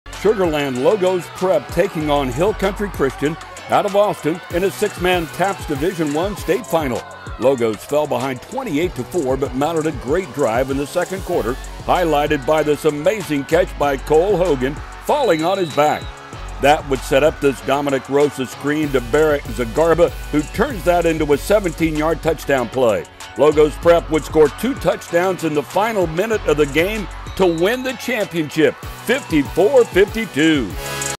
Logos Prep vs Hill Country Christian - 2022 Week 15 Football Highlights
Sugarland Logos Prep taking on Hill Country Christian out of Austin in a 6-Man TAPPS Division 1 state final

Logos fell behind 28-4, but mounted a great drive in the second quarter highlighted by this amazing catch by Cole Hogan falling on his back.

That would set up a Dominic Rosa screen to Barrett Zgarba (Zuh-garba) who turns that into a 17 yard touchdown.   Logos Prep would score 2 touchdowns in the last minute of the game to win the championship 54-52